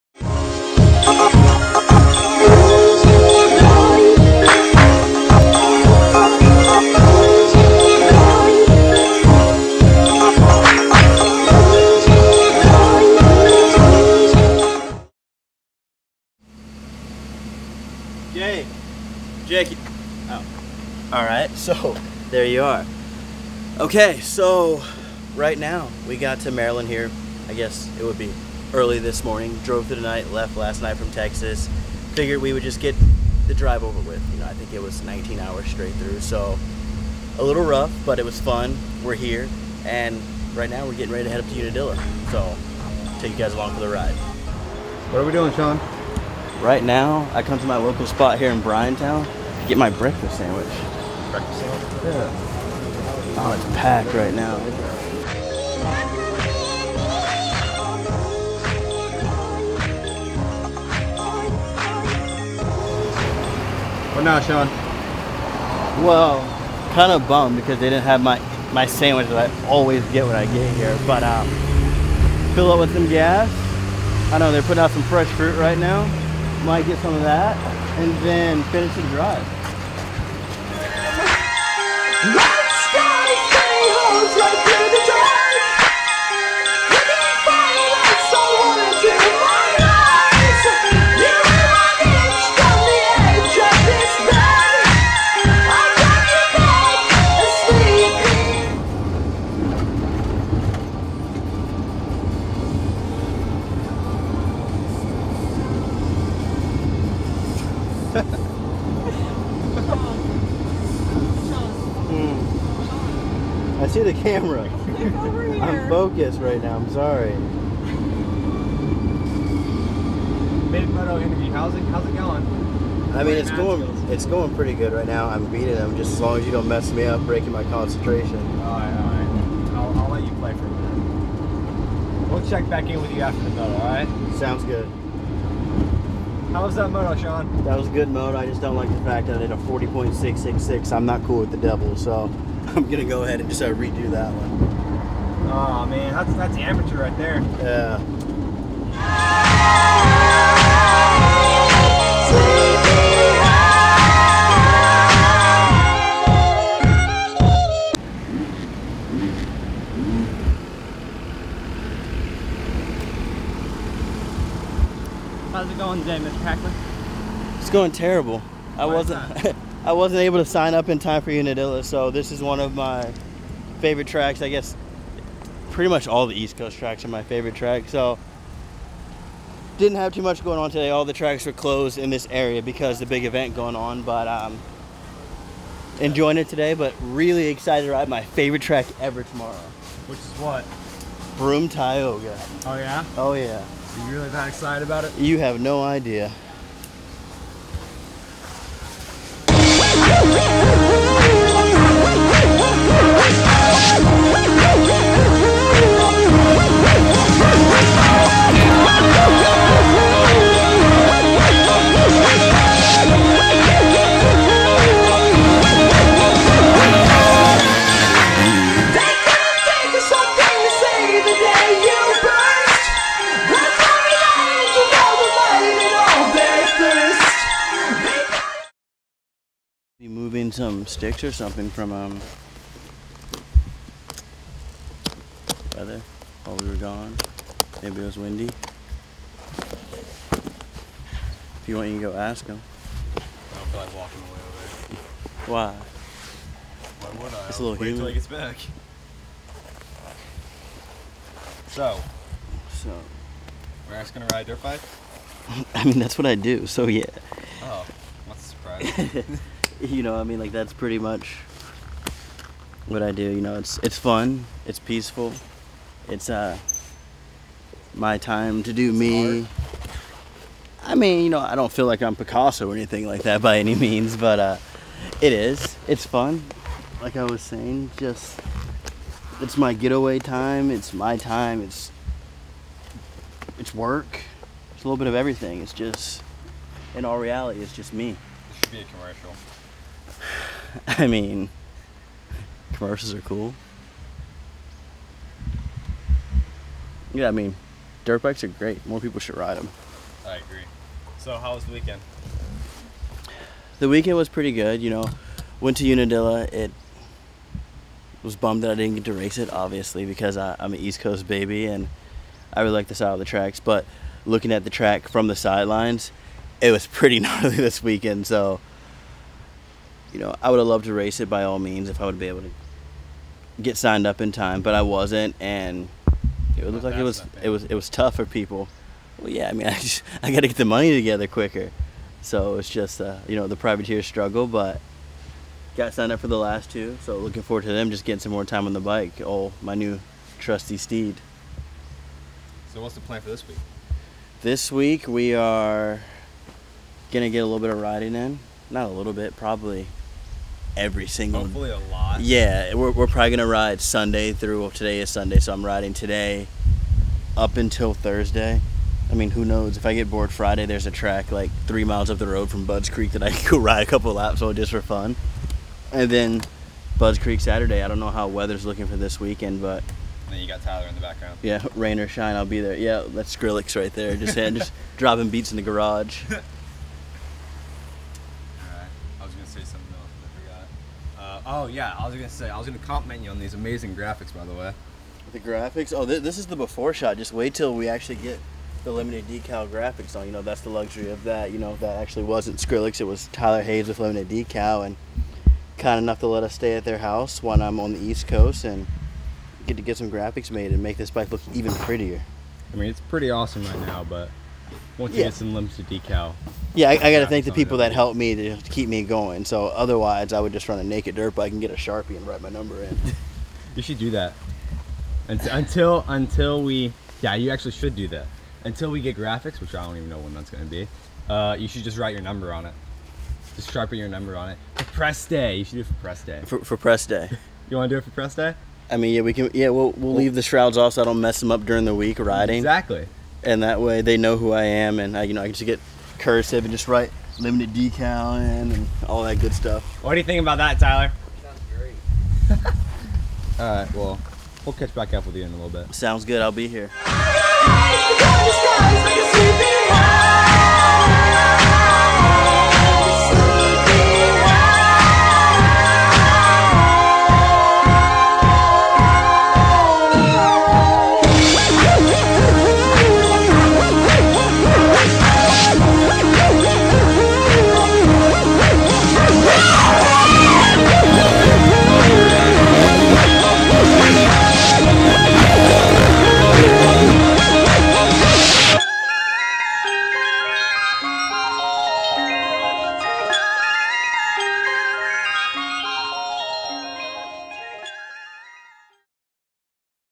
Finding Moto Episode 1 (Una-F'N-Dilla!!!)
From State to State, Driving, 1 bike, just given'r. Sean Hackley takes you with him as he travels the north east riding at local spots and racing the last three pro nationals. Sadly, the first part of the trip didn't go as planned. We were unable to ride at Broome Tioga but thats not stopping us!